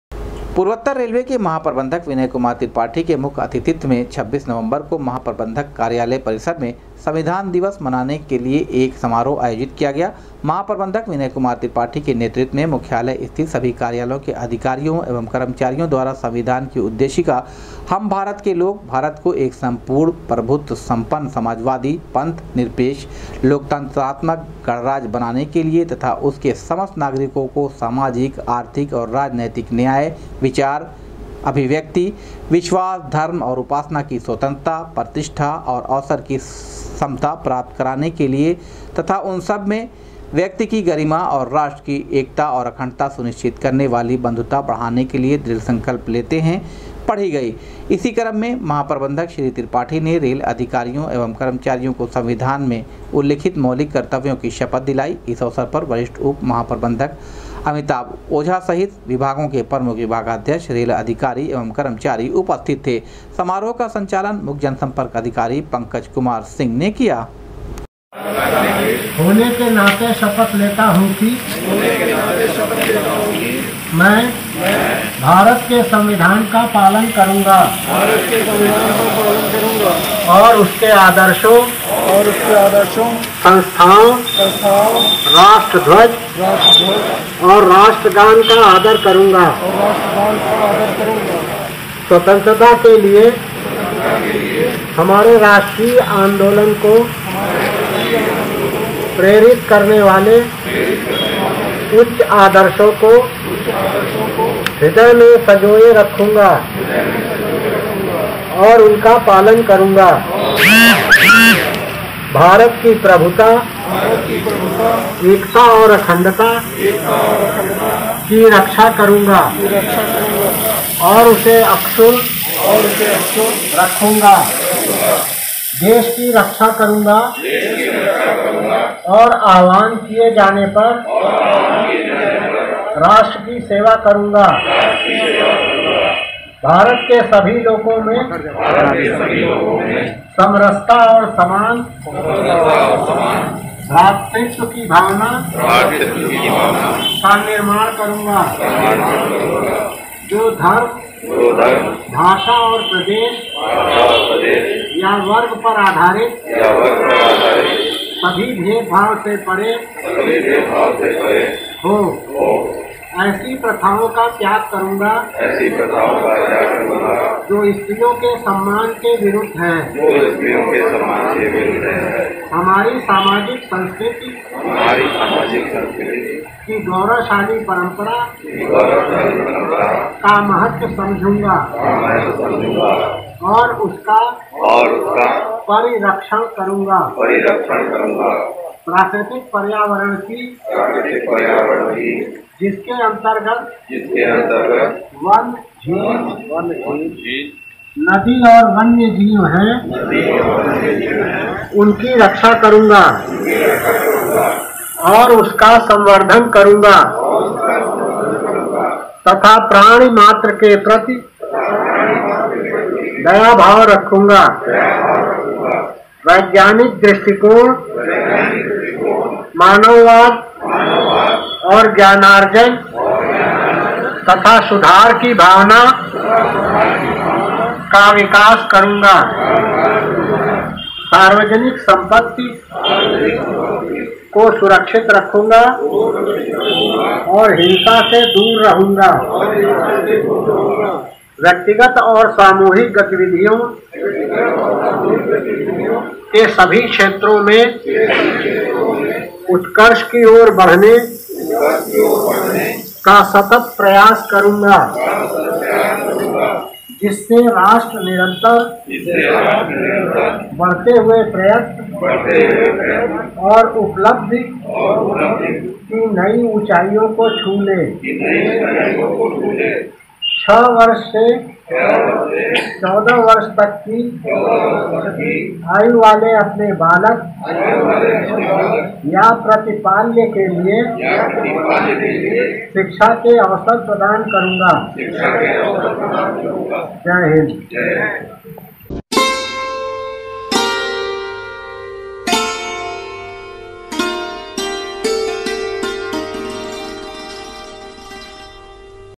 ’’संविधान दिवस’’ पर पूर्वोत्तर रेलवे के महाप्रबन्धक विनय कुमार त्रिपाठी ने रेल अधिकारियों एवं
’’संविधान दिवस’’ पर पूर्वोत्तर रेलवे के महाप्रबन्धक विनय कुमार त्रिपाठी ने रेल अधिकारियों एवं कर्मचारियों को संविधान में उल्लिखित मौलिक कर्तव्यों की दिलायी शपथ